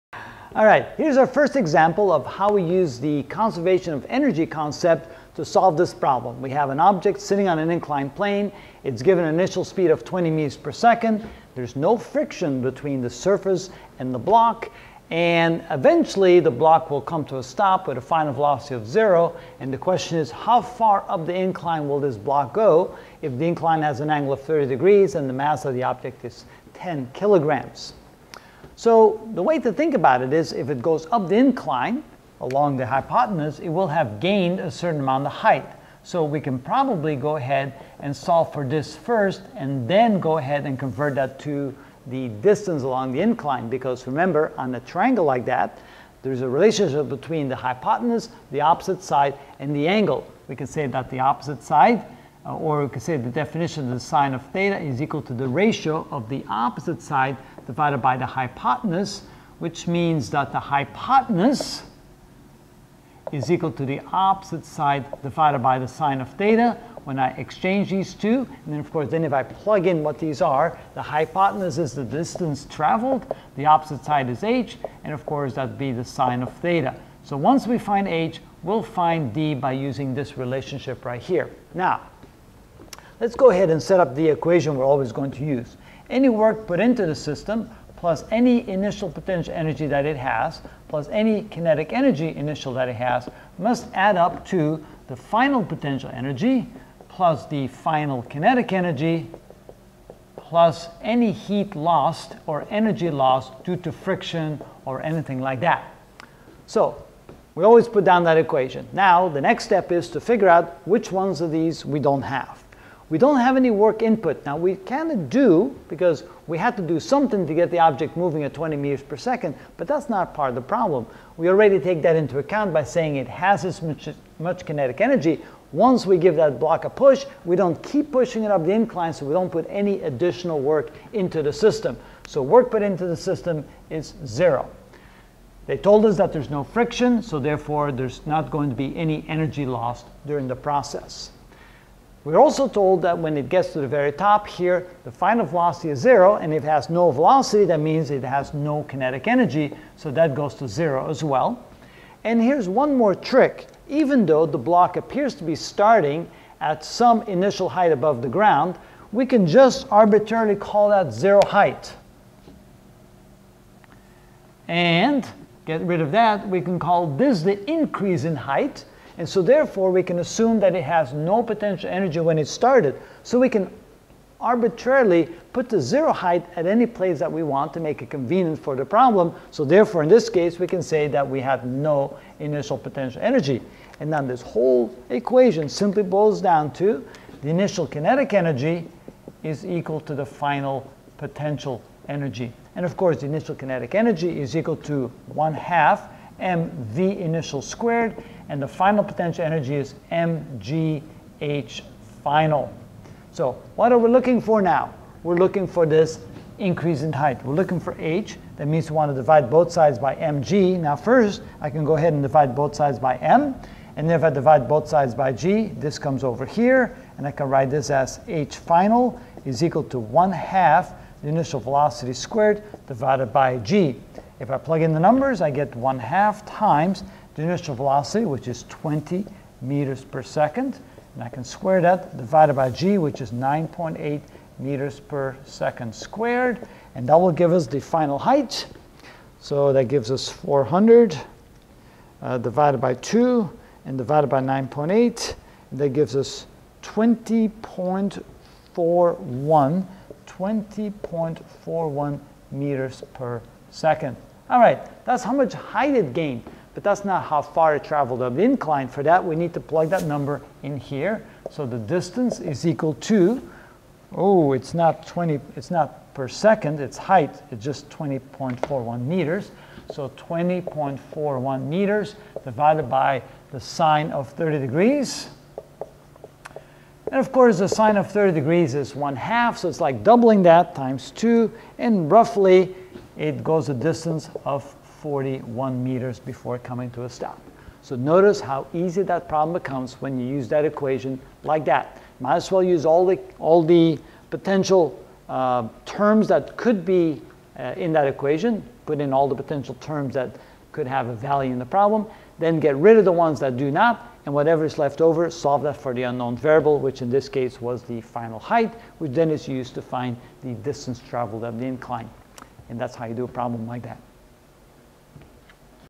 Physics 9 Conservation of Energy (2 of 11) Moving Up An Incline (Frictionless)
In this video I will show how to calculate the distance an object will travel up a (frictionless) incline.